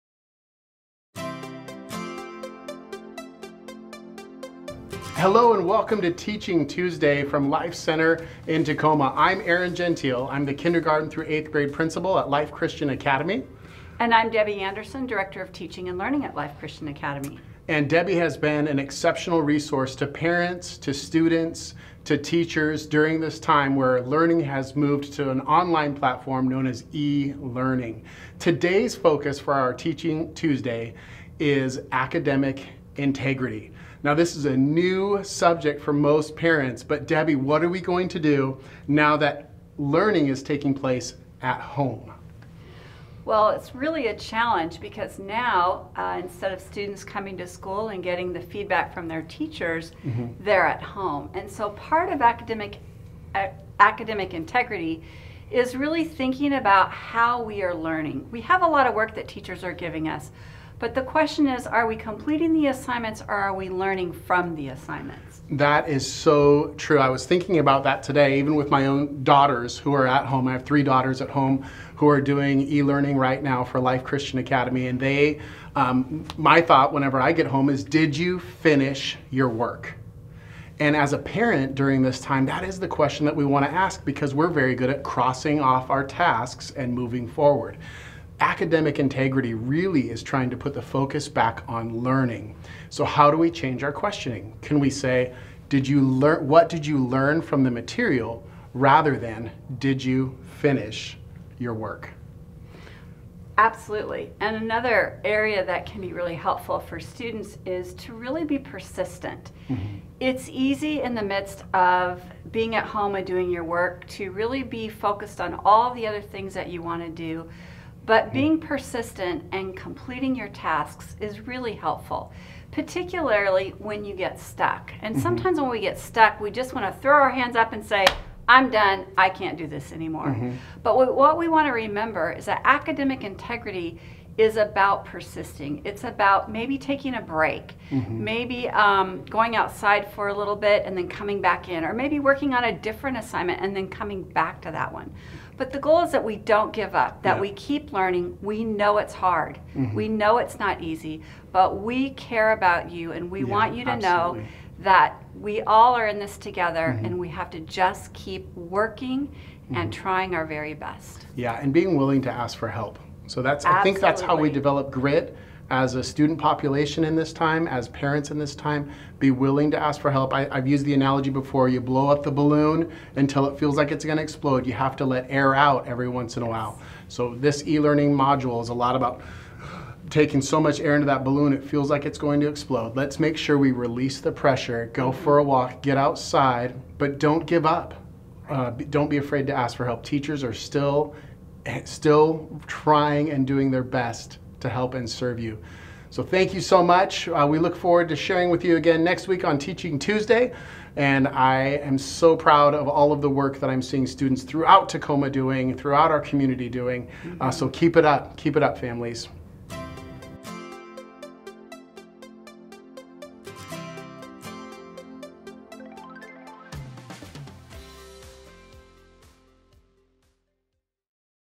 Teaching Tuesday | April 21st | Academic Integrity
LC On Demand Presents Teaching Tuesday with Aaron Gentile and Debbie Anderson. April 21st, 2020.